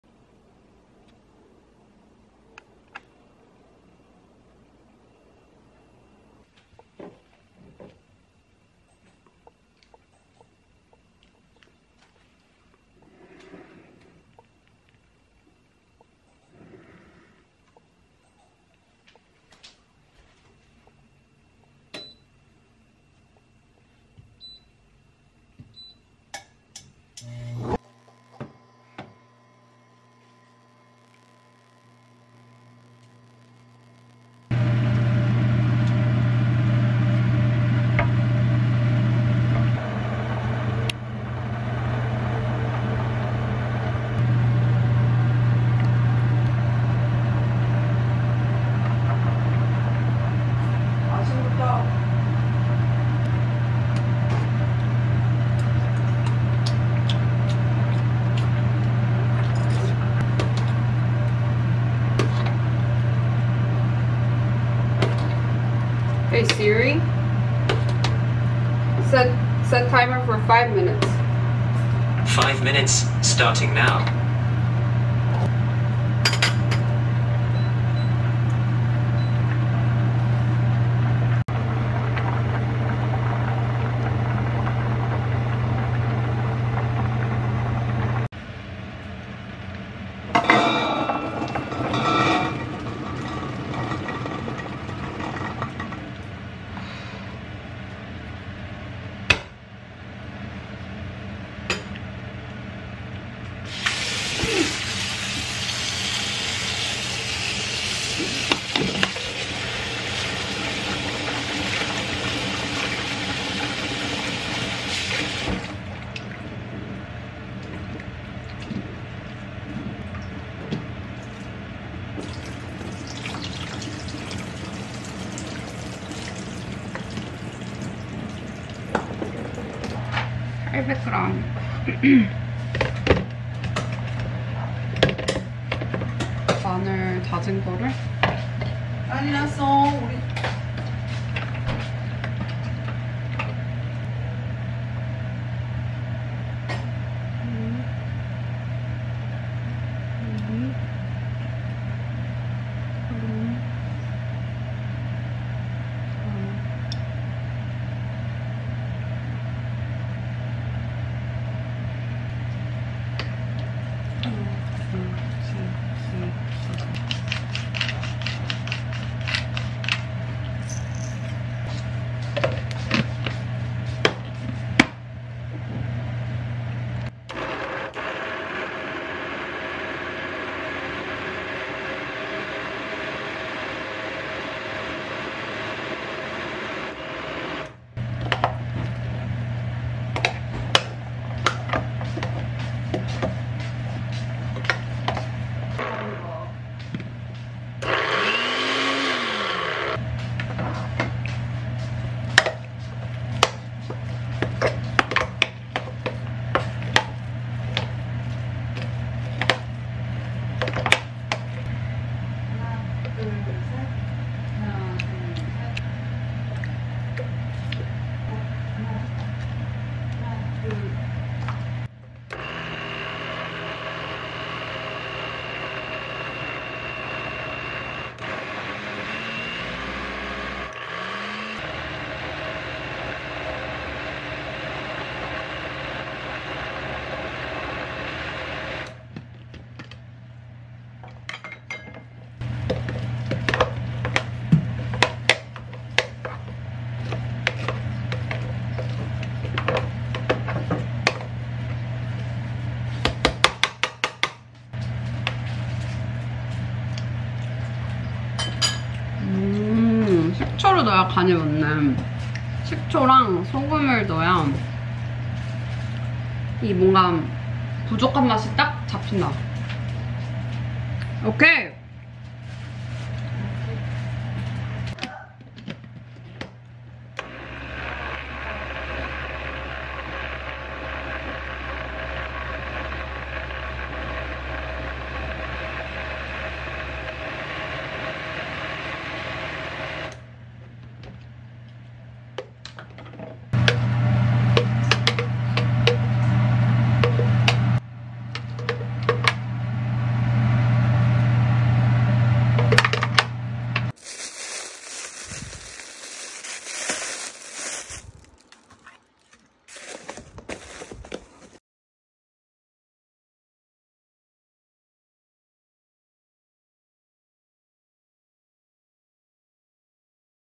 다이어트하는 의사 | 식단 공유 | 내가 다이어트 성공한 비결 | 후무스 만들기
"내가 다이어트 성공한 비결" 시리즈입니다. 다이어트는 무조건 식단입니다.